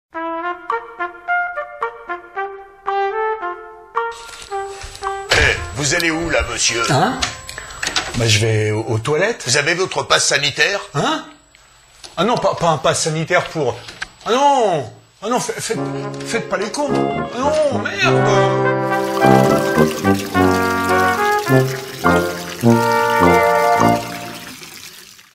(Pass sanitaire) Ca ne va pas être possible !!
Quand tu es dans la galère à cause de se foutu pass sanitaire covid.

Merci d'avance  ;-)